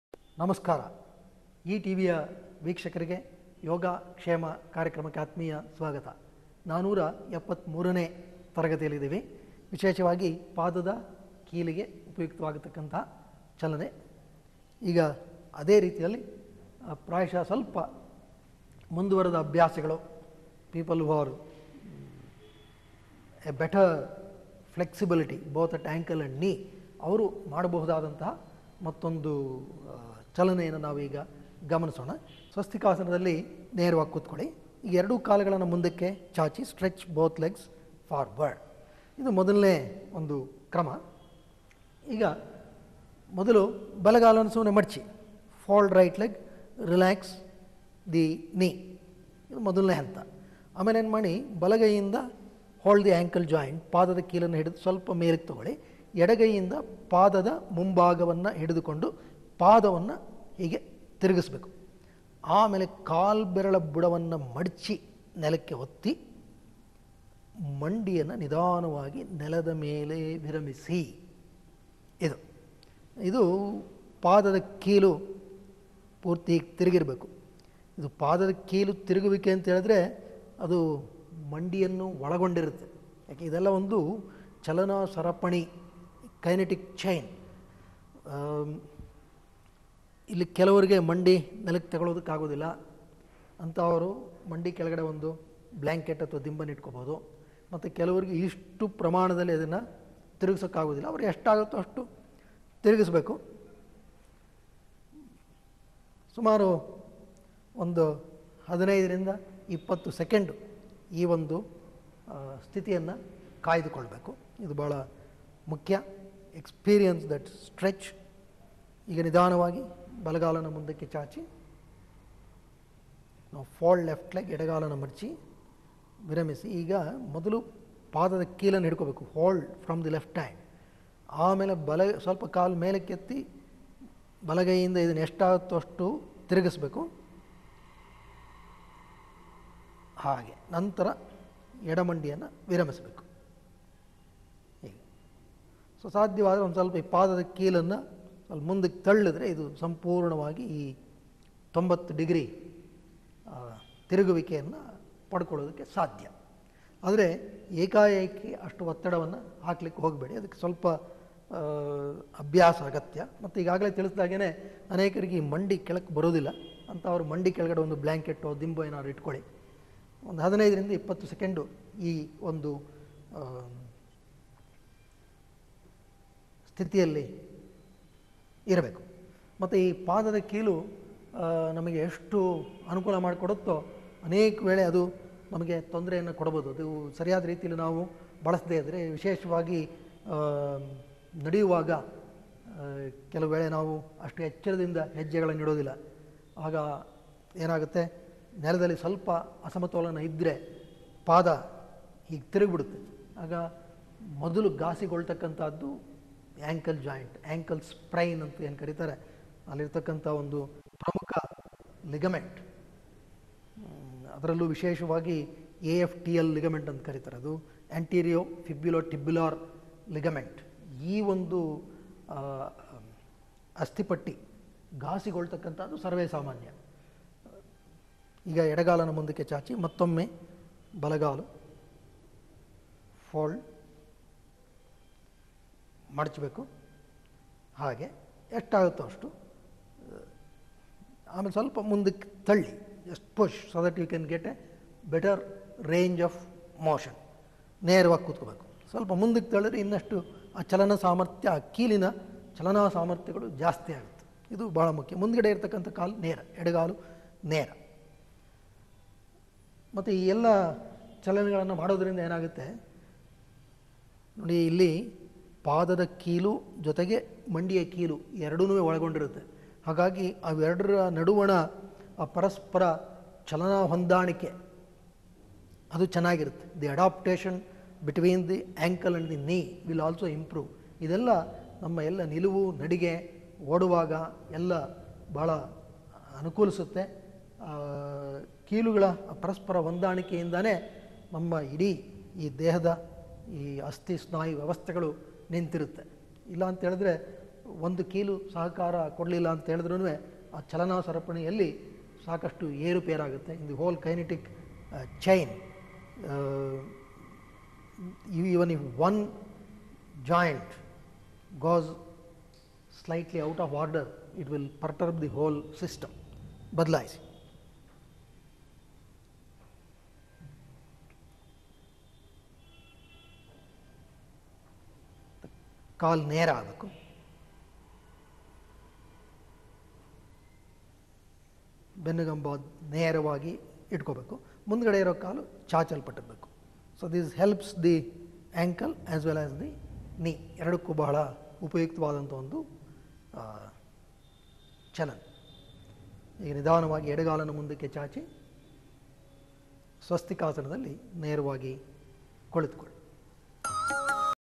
473 Ankle exercise with folding the ankle
473 Ankle exercise with folding the ankle & placing it next the thigh facing down